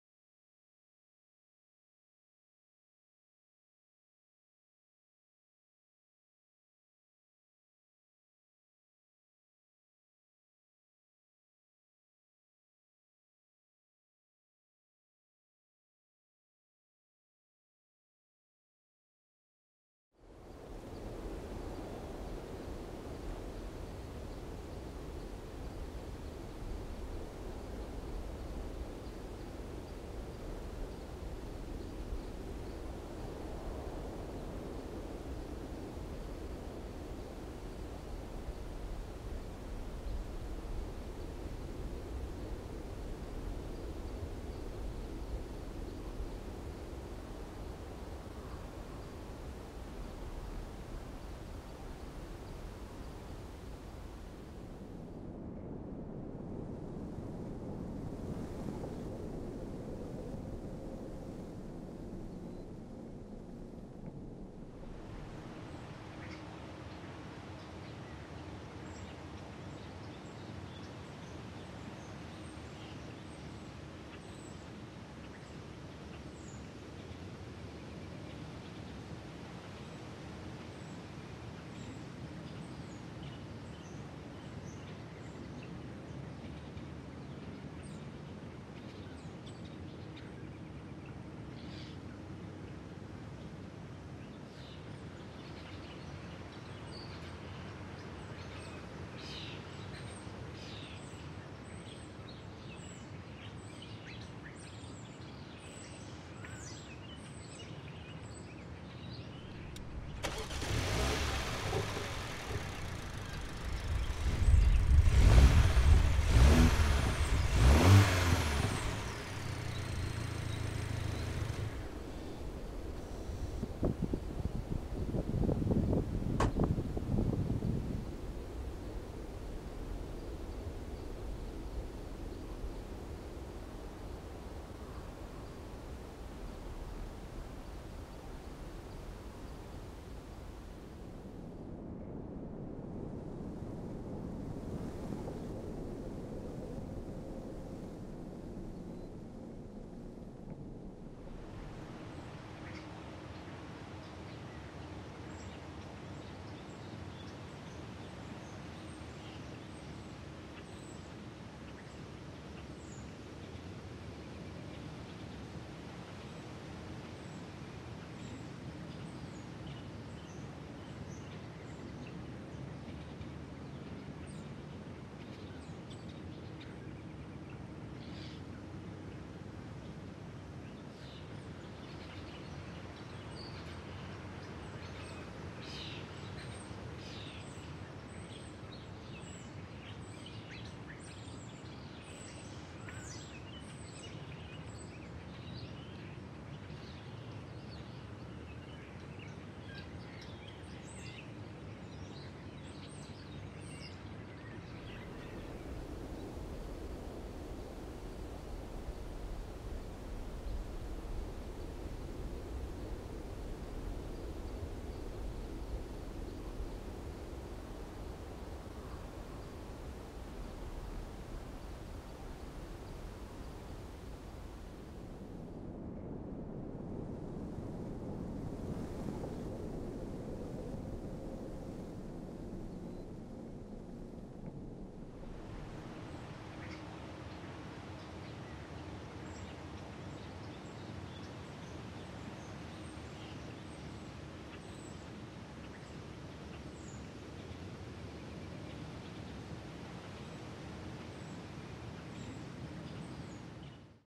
2020 BMW M135i xDrive video debut
The new 302-horsepower engine powering the M135i xDrive is the most powerful four-cylinder unit in the BMW Group line-up. The BMW M135i xDrive dispatches 0 to 100 km/h (62 mph) in a mere 4.8 seconds (4.7 seconds with M Performance package) on the way to a governed top speed of 250 km/h (155 mph).